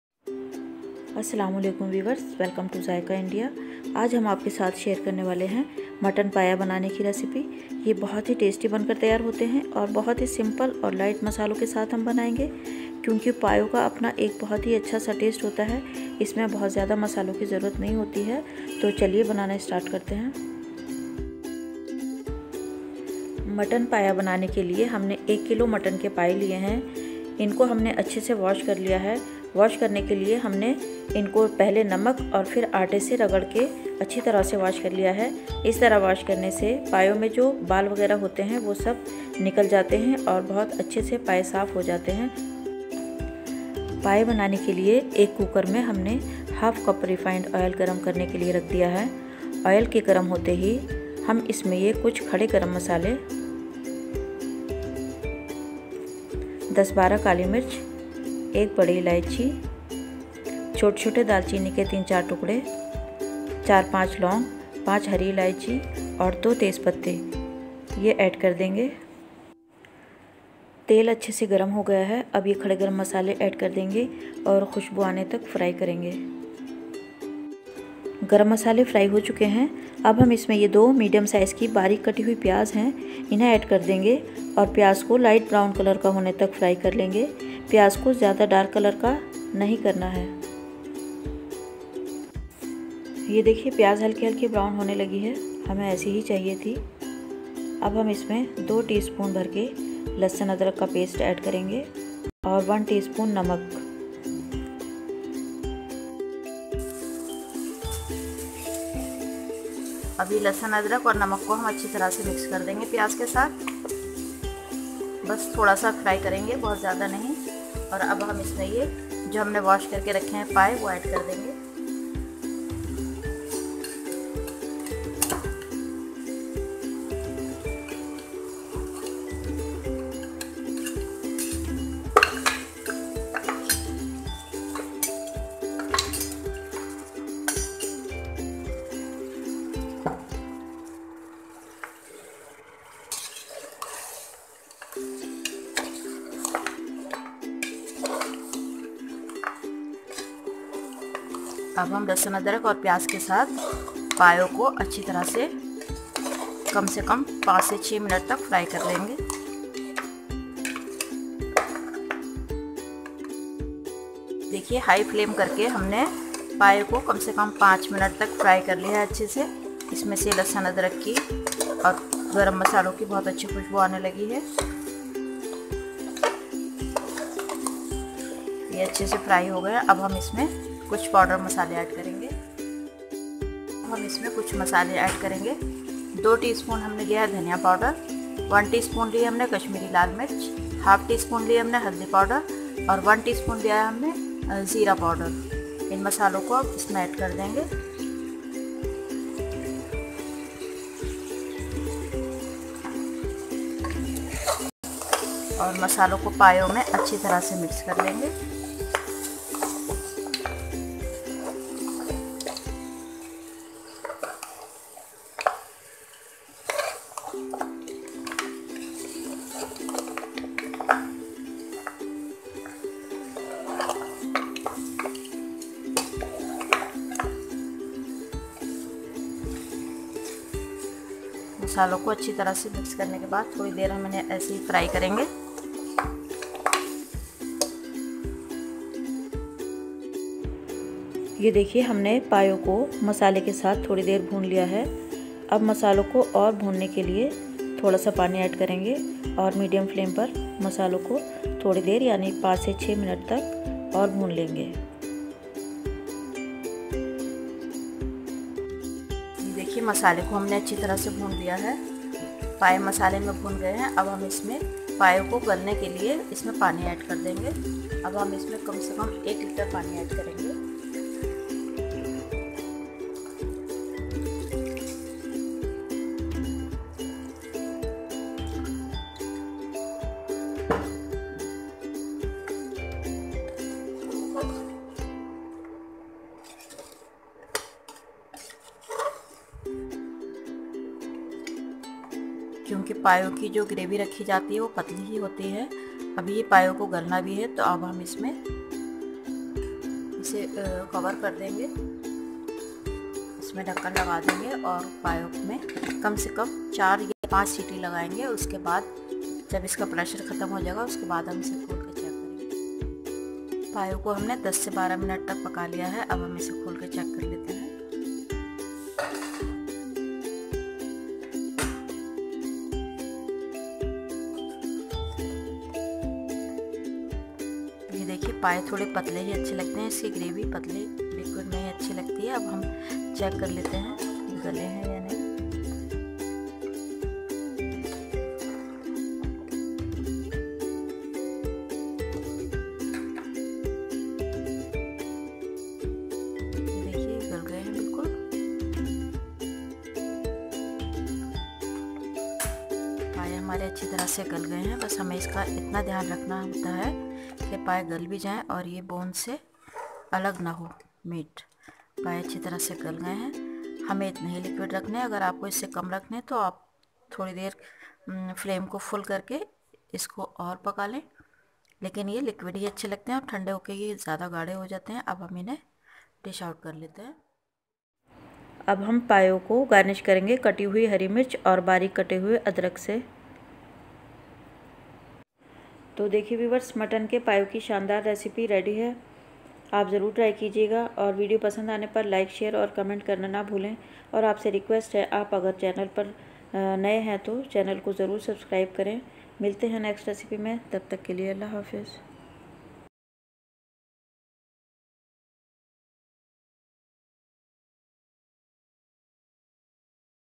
Mutton paya ||Trottters recipe || Cook with Azma
Ingredients :

1 kg Mutton paya
1/2 cup oil
10-12 black pepper
1 Black Cardamom
4-5 cloves
5 Green cardamom
2 bay leaves

2 medium sized onion
2 tsp Ginger-garlic paste

Powder spices

1 tsp salt
1/2 tsp Turmeric powder
1 tsp Kashmiri red chilli powder
1 tsp zeera powder
2 tsp Corriander powder

few Ginger slices and Chopped green chillies for garnishing